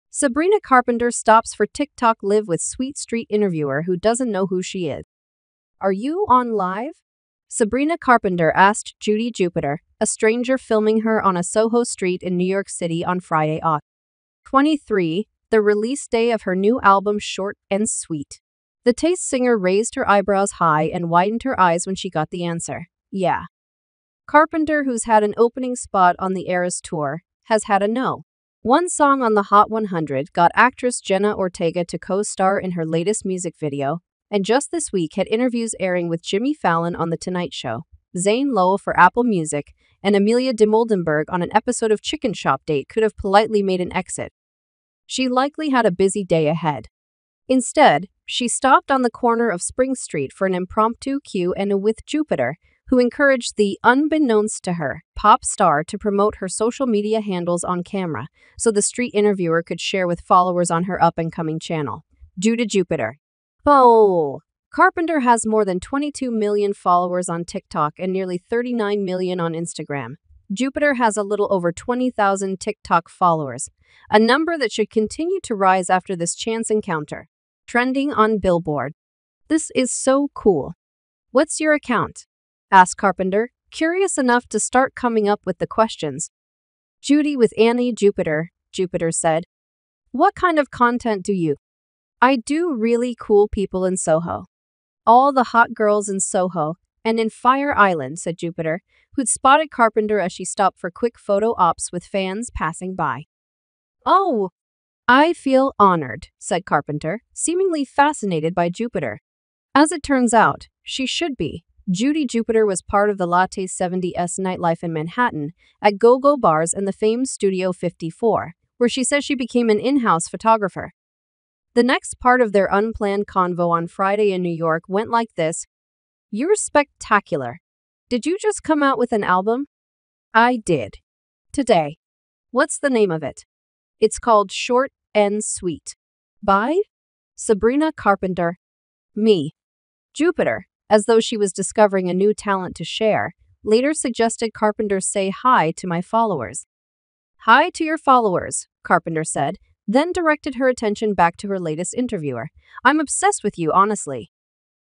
Sabrina Carpenter Stops for TikTok Live With Sweet Street Interviewer Who Doesn’t Know Who She Is
Sabrina Carpenter stopped for a TikTok live interview with Judi Jupiter on Soho street in New York City. Despite having an opening spot on The Eras Tour, a No. 1 song on the Hot 100, and co-starring with Jenna Ortega, Carpenter was able to engage with Jupiter, who encouraged her to promote her social media handles on camera. Carpenter has over 22 million followers on TikTok and 39 million on Instagram.